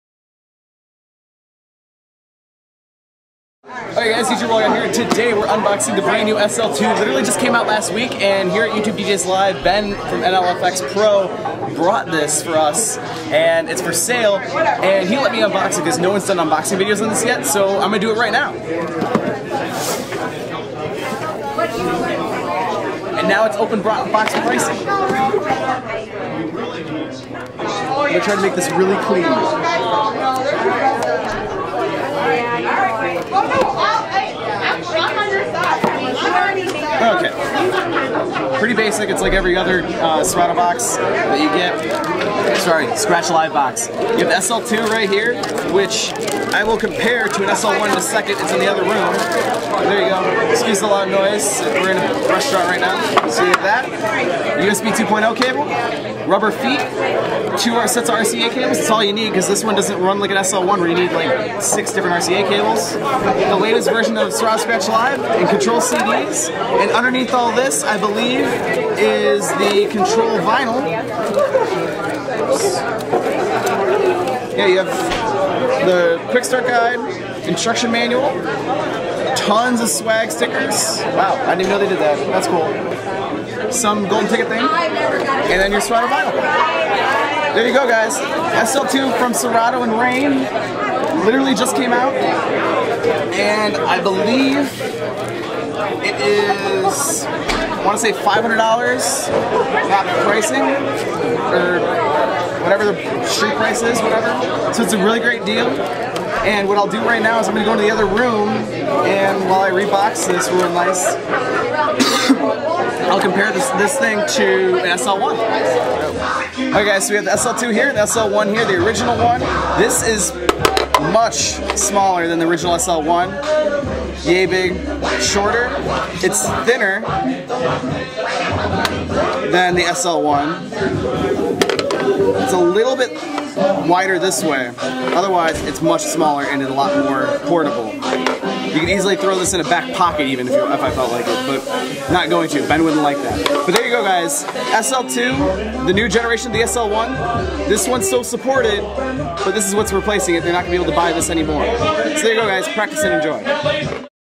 Rane SL2 Unboxing
Unboxing of the BRAND NEW Rane SL2! Sadly this isn't mine, but huge thanks to Ben Stowe of NLFX for allowing me to open this one for you guys!